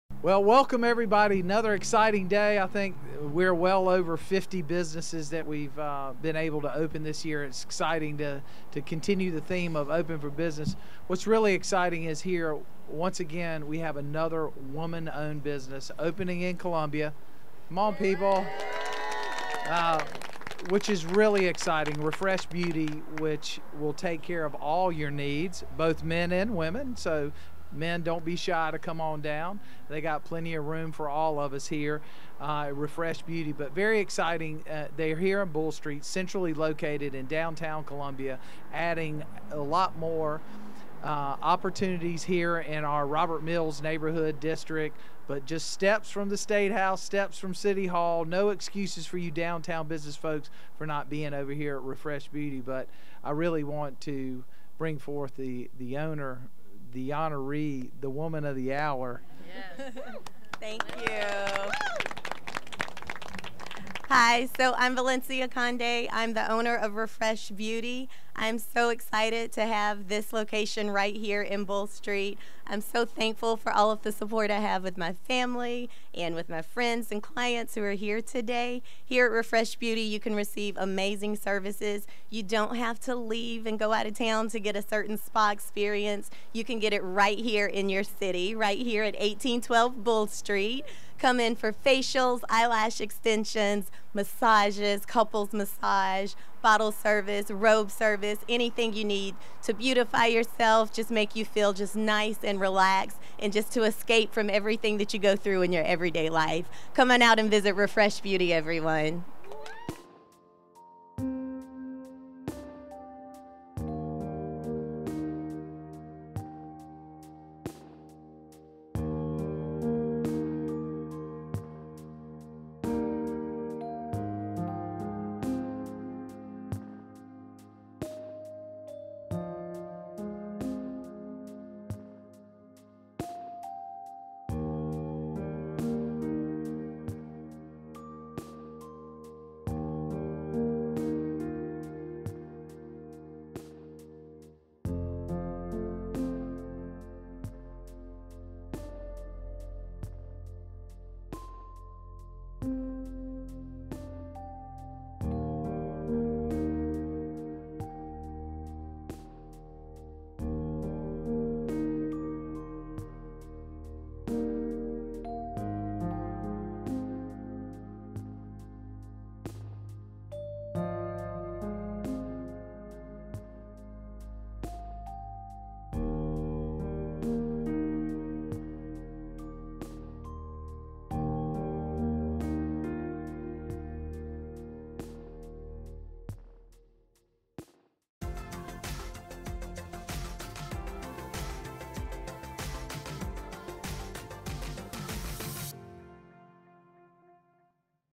Ribbon Cutting for Refreshed Beauty
Mayor Daniel J. Rickenmann helped cut the ribbon for another business located in the City of Columbia, Refresh Beauty at 1812 Bull St.

Refreshed Beauty is a locally owned wellness spa with expert therapists and service providers. Services include eyelash extensions, facials and massages.